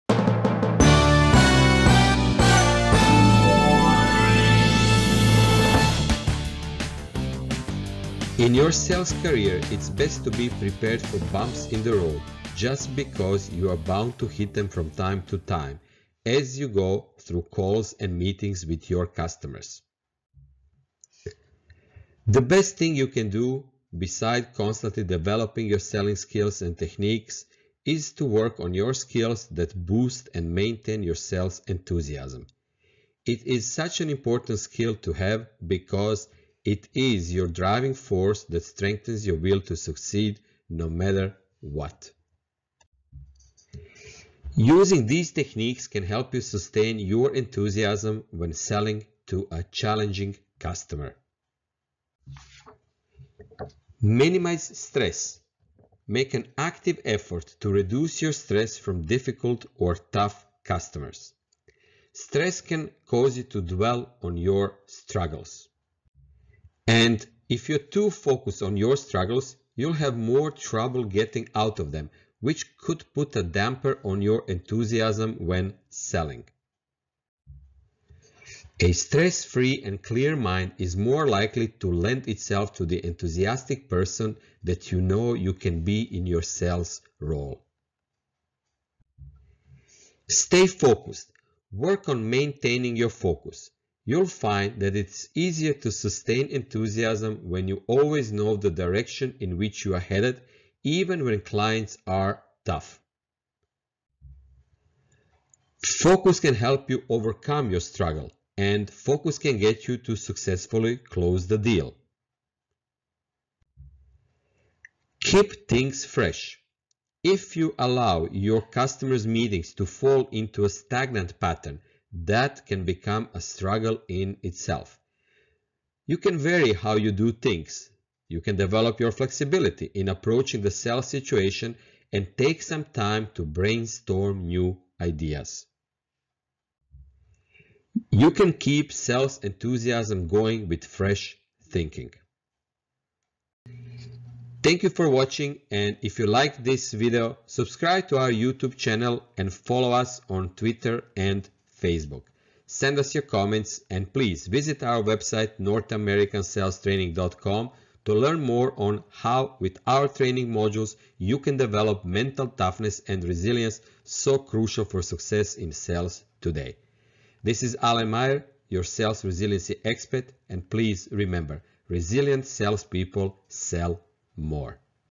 7 Ways to Maintain Your Sales Enthusiasm Despite Obstacles - Techniques #2, #3, #4
- How to include motivation maintenance into your everyday routines?
It's a piece of cake to maintain your enthusiasm when everything's rosy and peachy. When challenges in the boardroom arise, it can become increasingly difficult to stay motivated and continue progressing with your sales process.

Learn different strategies that will help you maintain your motivation during tough times with your customers. Using these techniques can help you sustain your enthusiasm when selling to a challenging customer:
2. Minimize stress.
3. Stay focused.
4. Keep things fresh.